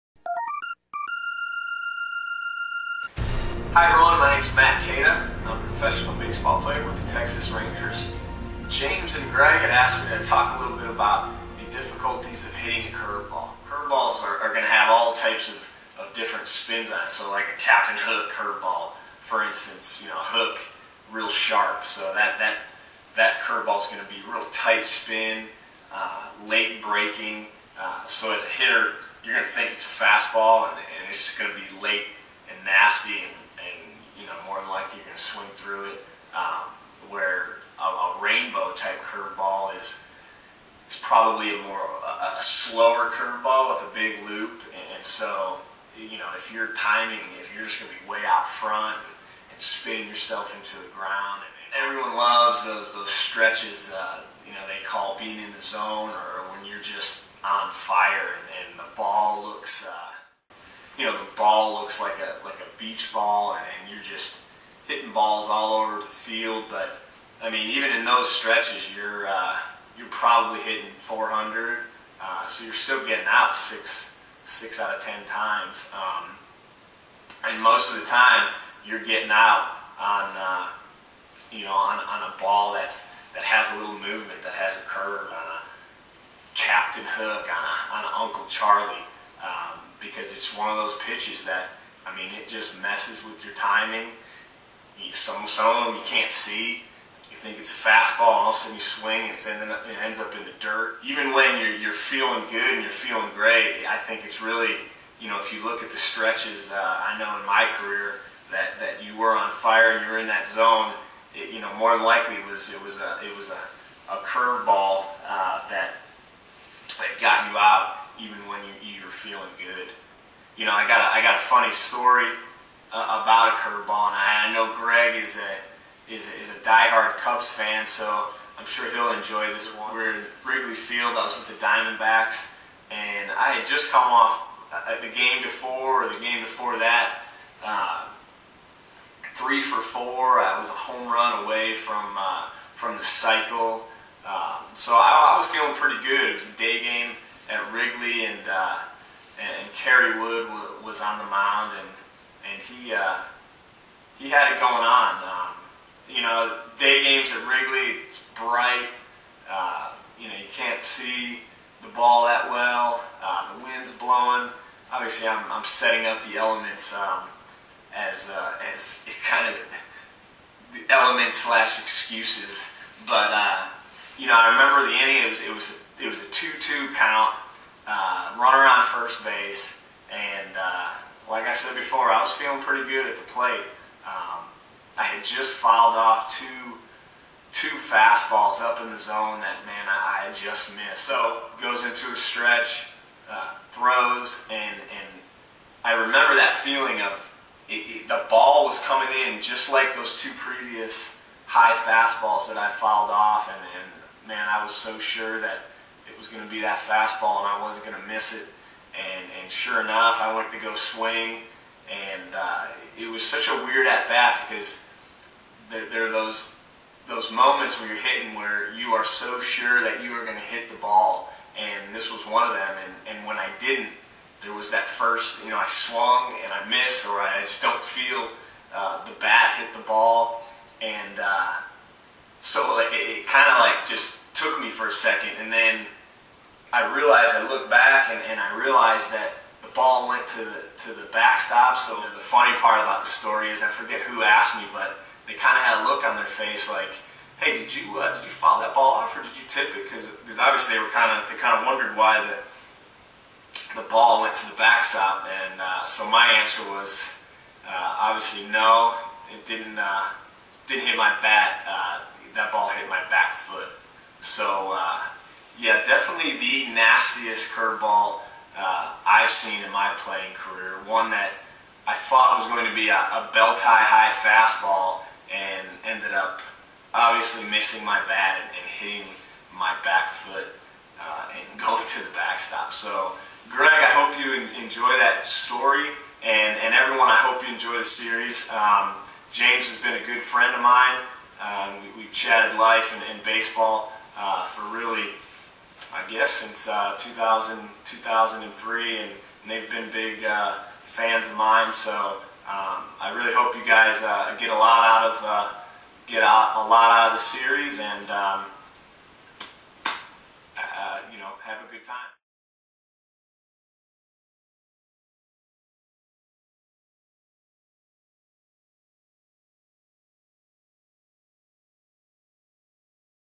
Matt Kata - The Curveball
speaking of baseball... this video, featuring former AZ Diamondback Matt Kata, was created last year for a series called "When Life Throws You A Curve". Can't believe I never posted it!  Cool insight from an athlete who has seen first-hand the challenge of hitting a curveball...